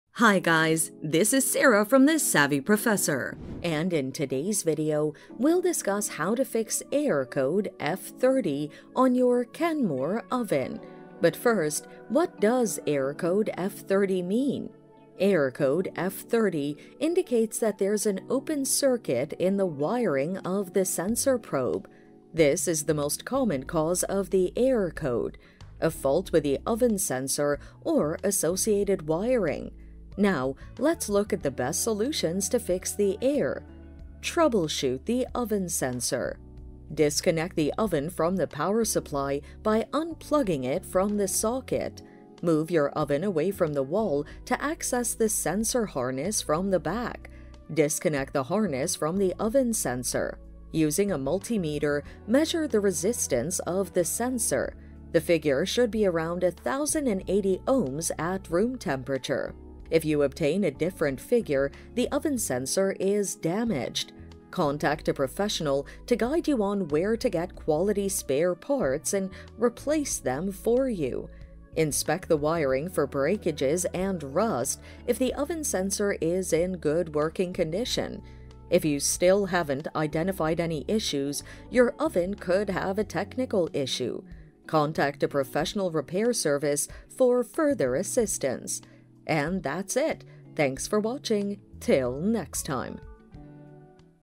Kenmore Oven Error Code F30 (How To Troubleshoot Error Code F30 - Ultimate Guide!)
Kenmore Oven Error Code F30 (How To Troubleshoot Error Code F30 - Ultimate Guide!). In this video tutorial I will show you how to fix Kenmore Oven error code F30.

Error code F30 indicates that there is an open circuit in the wiring of the sensor probe.

This is the Most Common cause of the Error Code.
• Fault with the oven sensor or associated wiring.

Troubleshoot the Oven Sensor
• Disconnect the oven from the power supply by unplugging it from the socket.
• Move your oven away from the wall to access the sensor harness from the back.
• Disconnect the harness from the oven sensor.
• Using a multimeter, measure the resistance of the sensor. The figure should be around 1080 ohms at room temperature.
• If you obtain a different figure, the oven sensor is damaged.
• Contact a professional to guide you on where to get quality spare parts and replace them for you.
• Inspect the wiring for breakages and rust if the oven sensor is in good working condition.
• If you still haven’t identified any issues, your oven could have a technical issue.
• Contact a professional repair service for further assistance.